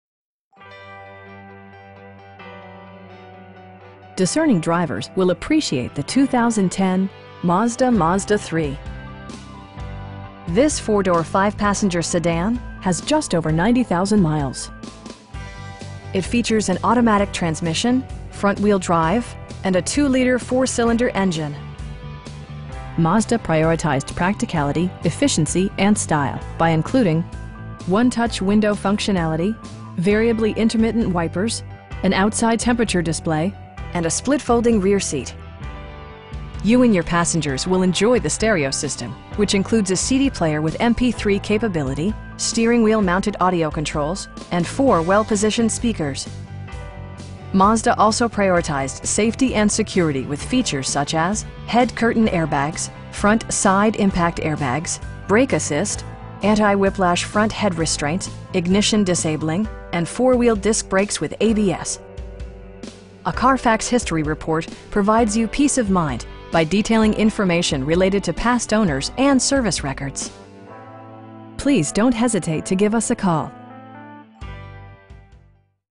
2010 Mazda Mazda3 i in Greensboro, NC 27407
3633 W. Wendover Ave

Discerning drivers will appreciate the 2010 Mazda Mazda3. This 4 door, 5 passenger sedan has just over 90,000 miles. It features an automatic transmission, front-wheel drive, and a 2 liter 4 cylinder engine. Mazda prioritized practicality, efficiency, and style by including: 1-touch window functionality, variably intermittent wipers, an outside temperature display, front bucket seats, power door mirrors, power windows, and a split folding rear seat. You and your passengers will enjoy the stereo system, which includes a CD player with MP3 capability, steering wheel mounted audio controls, and 4 speakers, providing excellent sound throughout the cabin. Mazda also prioritized safety and security with features such as: head curtain airbags, front SIDE impact airbags, brake assist, anti-whiplash front head restraints, ignition disabling, and 4 wheel disc brakes with ABS. A Carfax history report provides you peace of mind by detailing information related to past owners and service records. We pride ourselves in the quality that we offer on all of our vehicles. Please don't hesitate to give us a call.